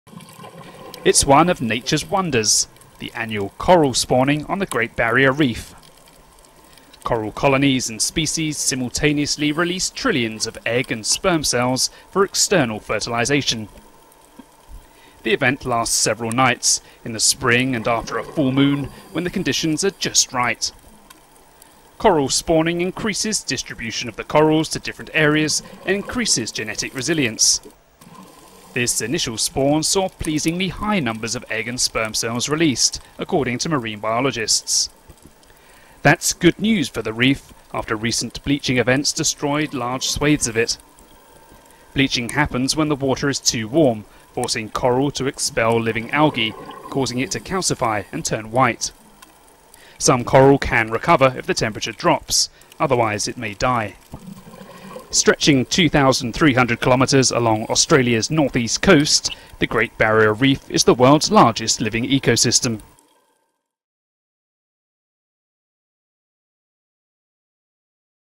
Coral spawning spectacle on the Great Barrier Reef
Underwater footage captures the moment future generations of coral reef are created on the Great Barrier Reef during recent coral spawning. The healthy spawning shows the healthy revival of coral after recent bleaching events destroyed large swathes of it.